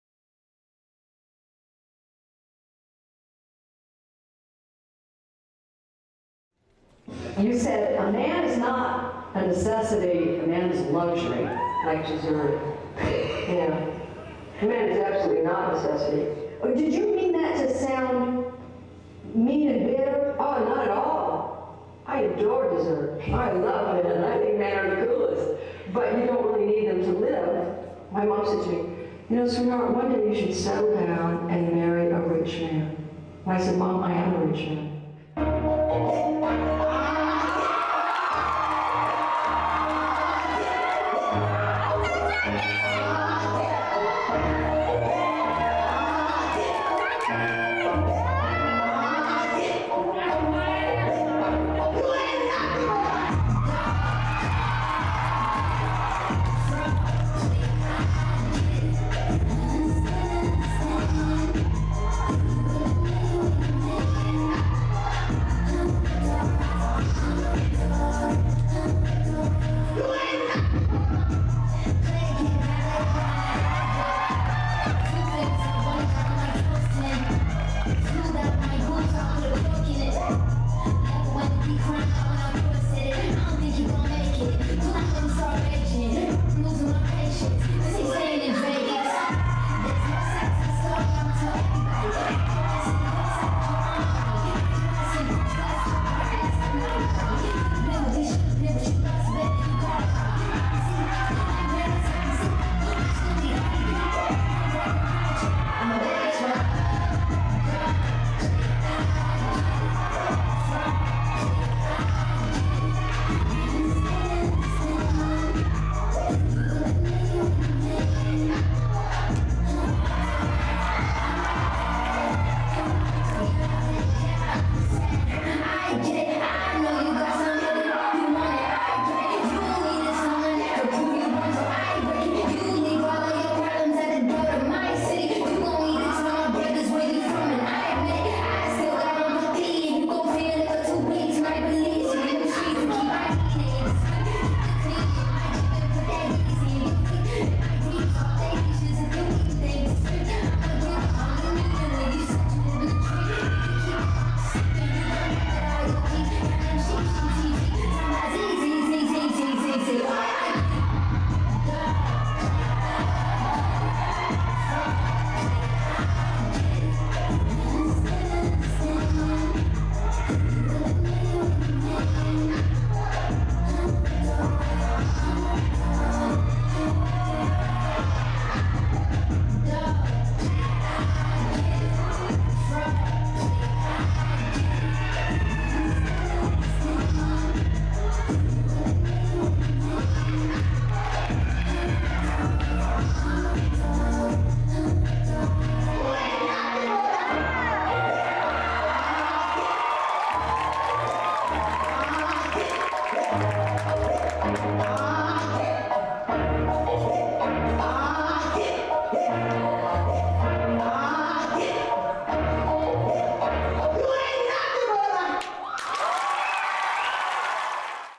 What Happens in Vegas
Choreographed by AV Justino-Isaac
Emerson Urban Dance Theatre
Fall 2022 Showcase: Cinema

Video Filmed/Created By: Rose Luczaj